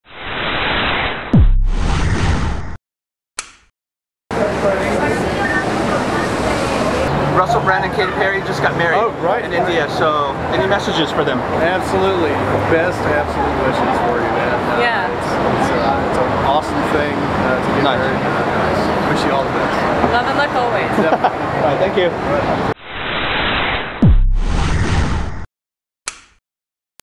JamesPitt & Emma Zerner @ Katsuya
James Pitt and Emma Zerner wish Katy Perry and Russel Brand love and luck always as they enter Katsuya restaurant!!